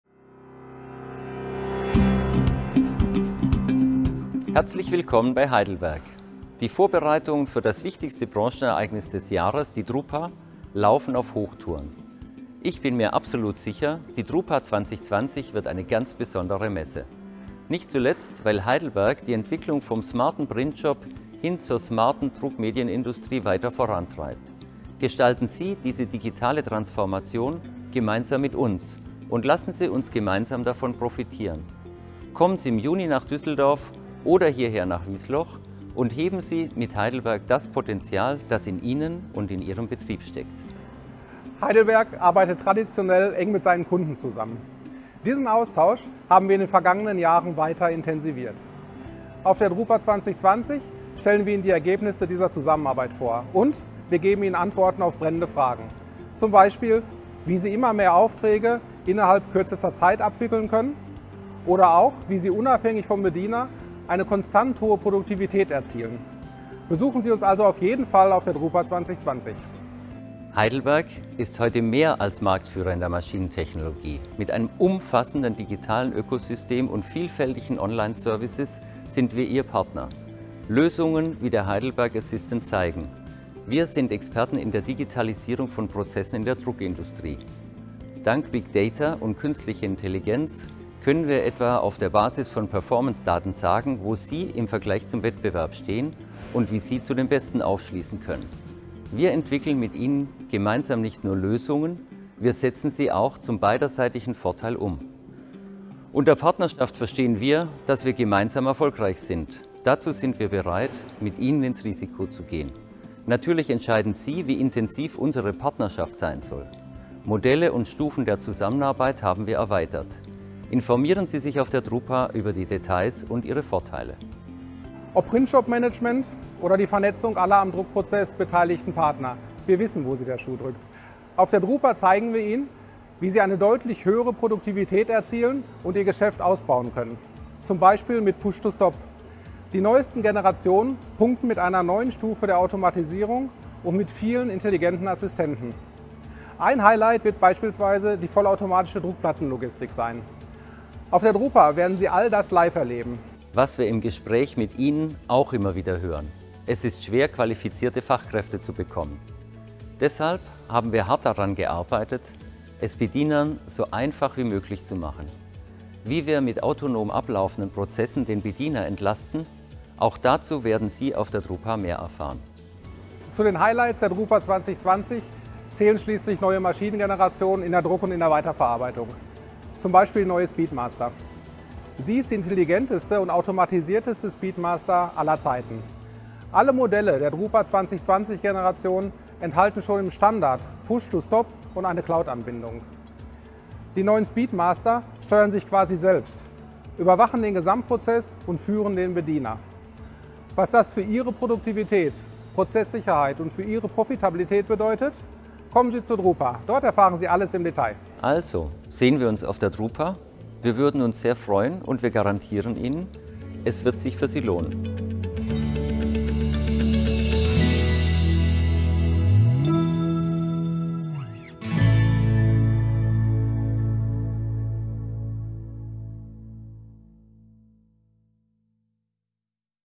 Experience Heidelberg up close and personal at drupa./Erleben Sie Heidelberg hautnah auf der drupa.
“Unfold your potential”, the Heidelberg motto for drupa 2020, is more than just a catchy slogan – it is a concept the company lives and breathes. Visit us this June in Düsseldorf and Wiesloch-Walldorf and discover how you can unlock the potential that comes from digitizing printing processes.